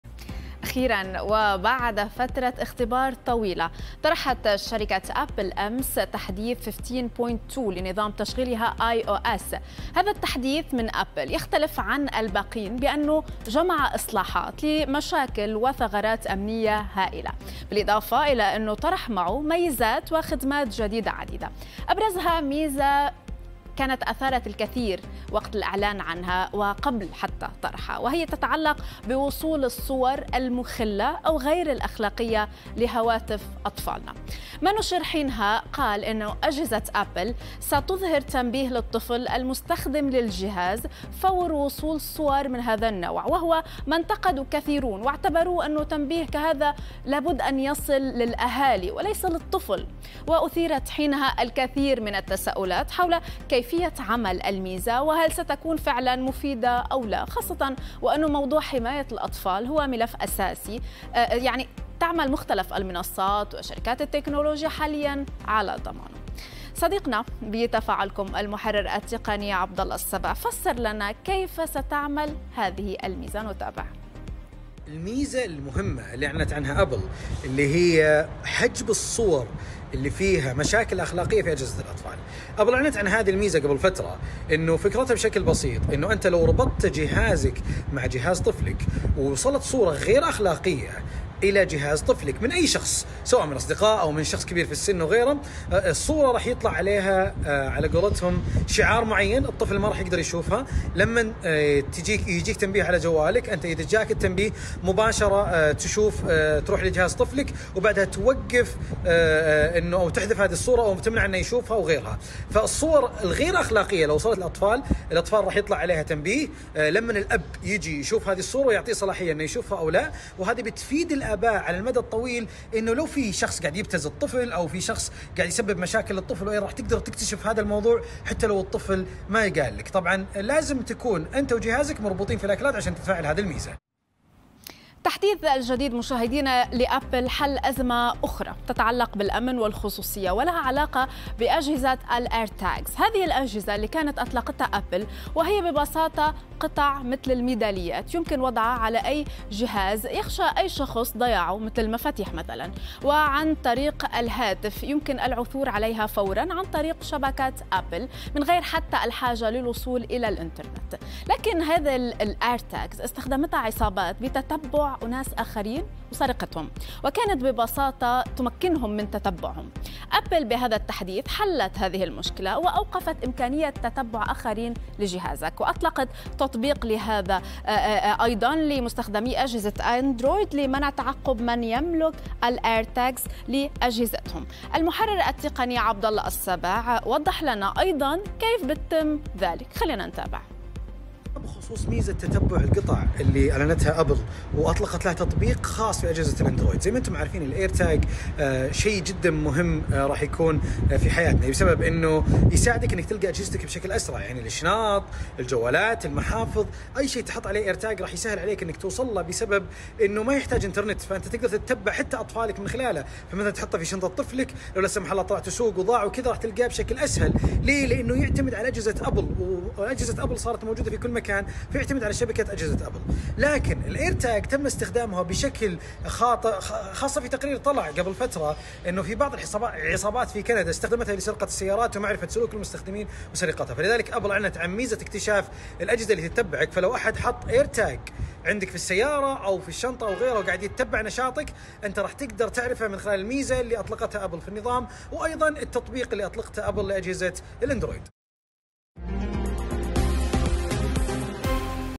تفاعلكم | حماية الأطفال ومنع التعقب.. أبرز ميزات تحديث أبل الأخير
شركة أبل تطلق تحديثا جديدا يمنع وصول الصور المخلة لهواتف الأطفال ويساعد المستخدمين على معرفة إن كان هناك أفراد يتعقبون أجهزتهم